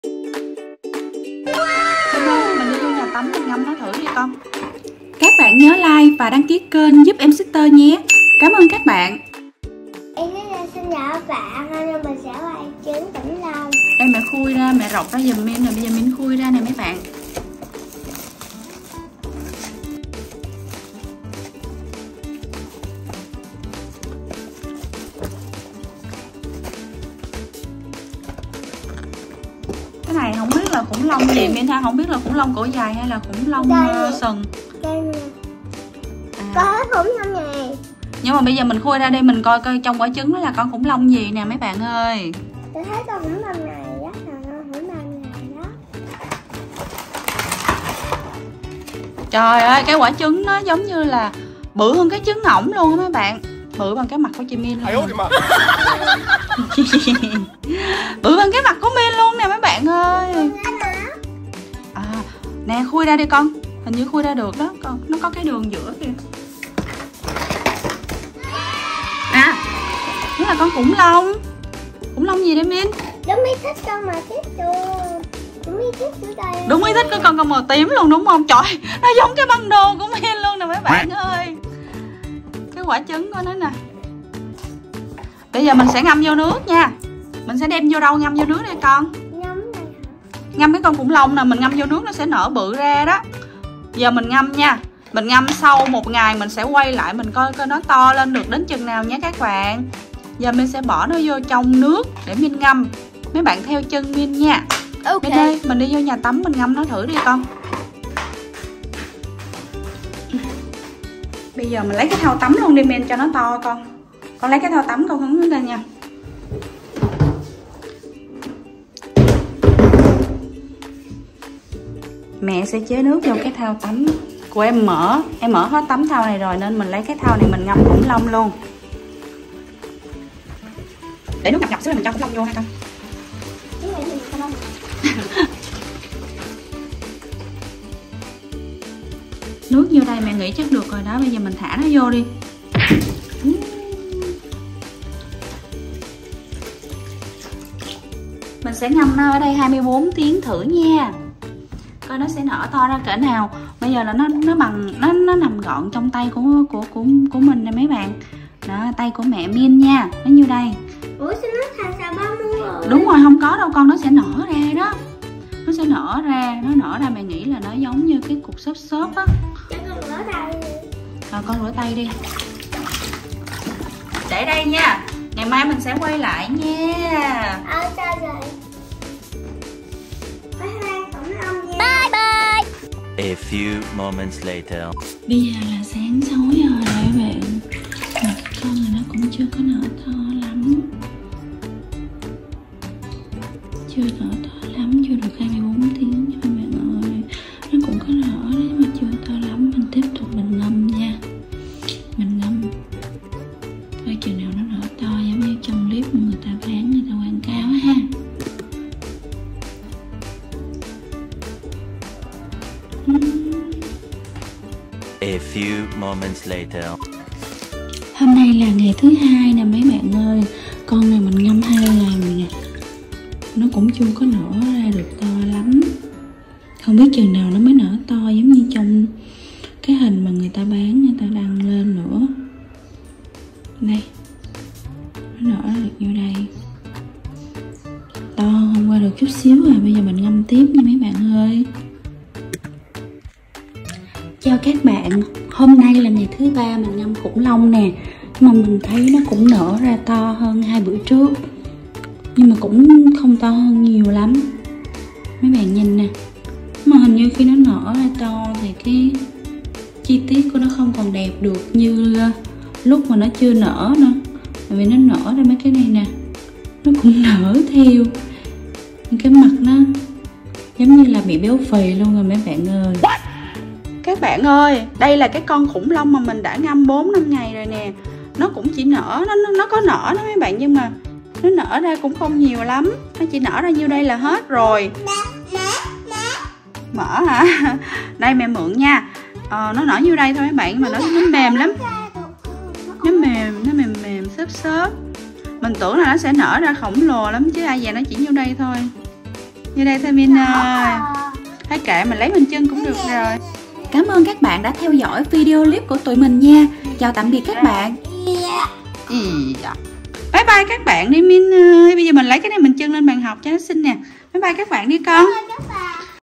Hatching Dinosaur eggs🦖🦕Trứng nở Khủng Long🥚
Đây là món quà sinh nhật Mint được tặng.
Chúng mình cùng khui ra xem quả trứng nở ra gì nhé các bạn.

Tks you